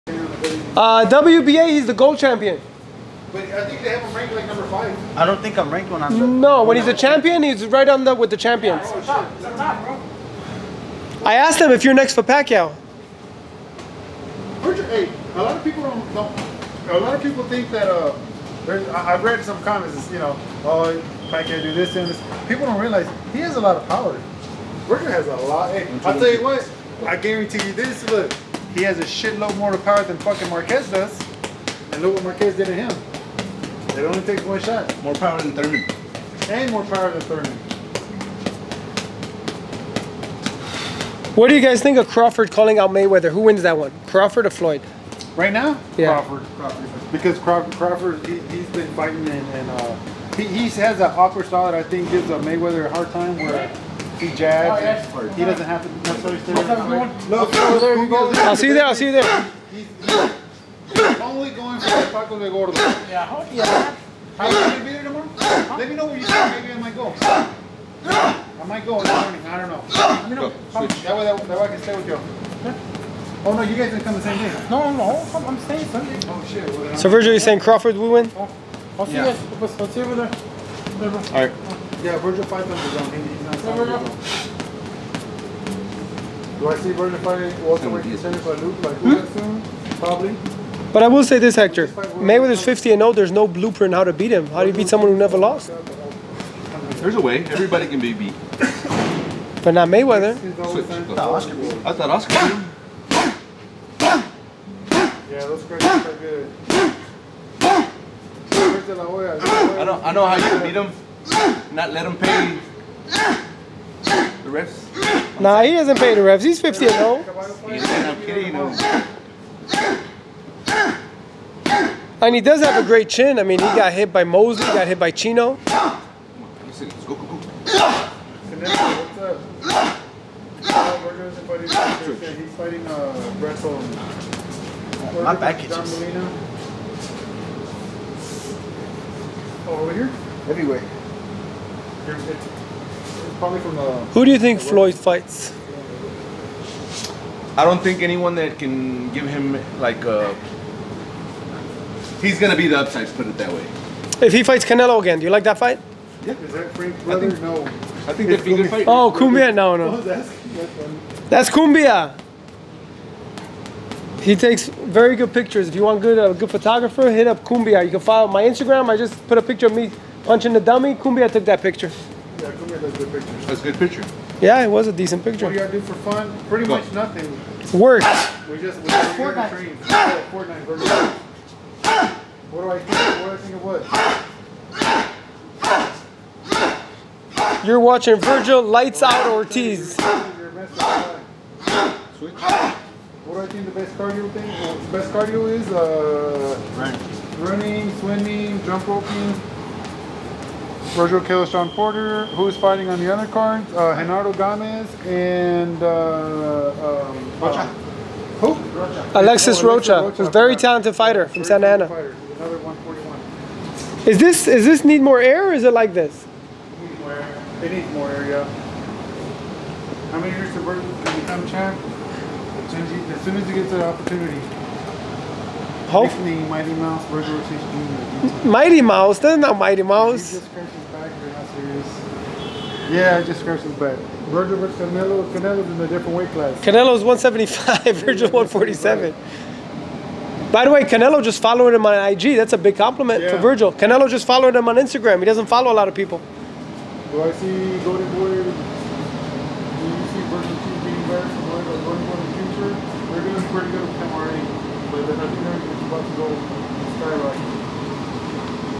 Vergil Ortiz 14-0, 14 KOs: Ready For Any 147lb Star - EsNews Boxing
Vergil Ortiz has an impressive record he's looking to extend on December 13 versus Brad Solomon. Think he'll manage it?